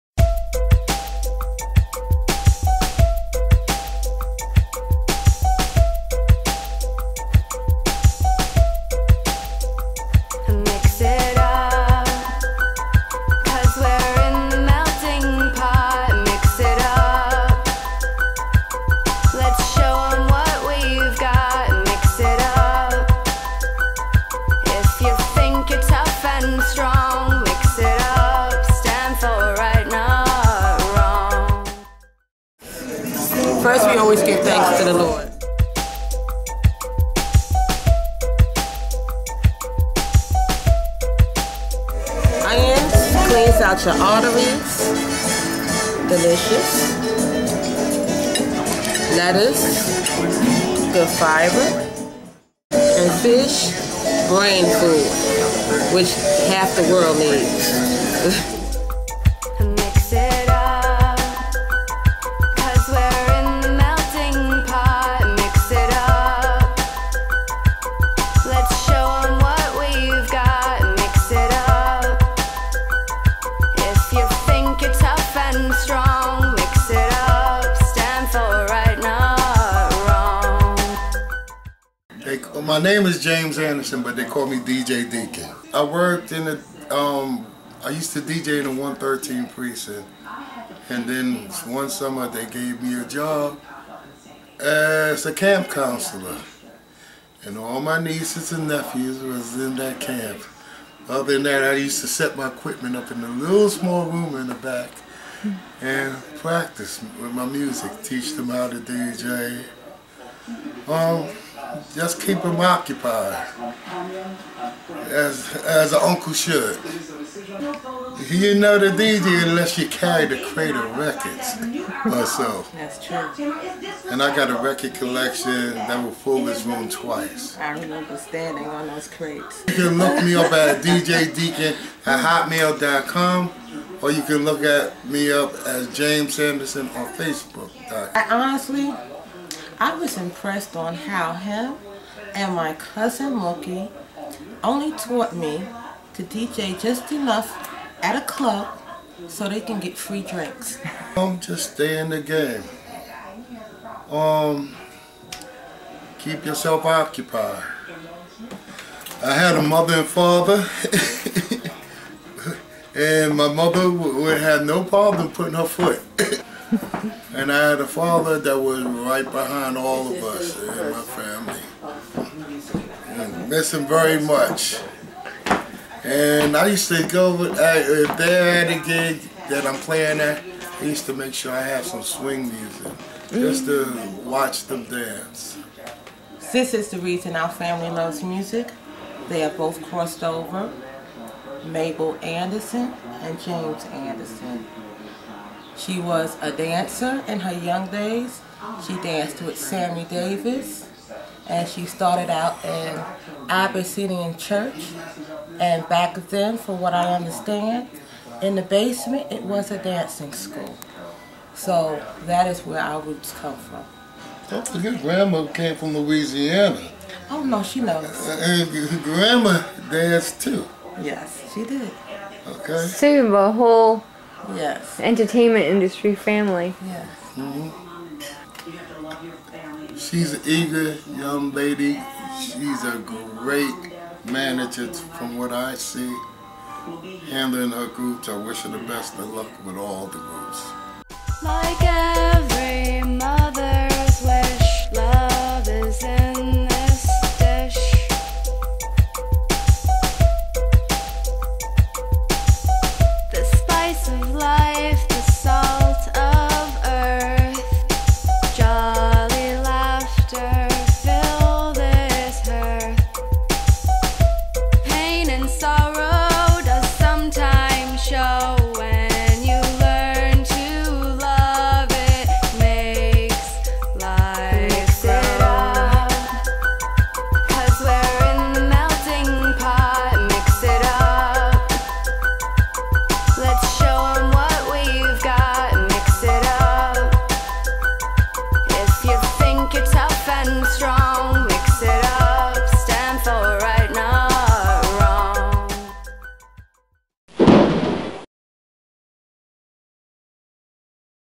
QUEENS NY 2012 Field Trip with Lucy
This is a mix of footage I shot on location in Queens New York September 2012.

Starring
Nicole Snider
Lucy Diamond

special guest James Anderson (aka DJ DEACON )

this is an ILN Production

created by Lucy Diamond
for Gemjin.com/tv

location coordinator James 'DJ DEACON' Anderson
Director Nicole Snider
Executive Producer ILN Productions
Digital Editing Director Lucy Diamond
Kraft services and Transportation provide by Amer I Can CLL
Music produced by Spaced Out Music Group, NY
Vocalist Kristina Theresa
Theme song written by Alison McDermott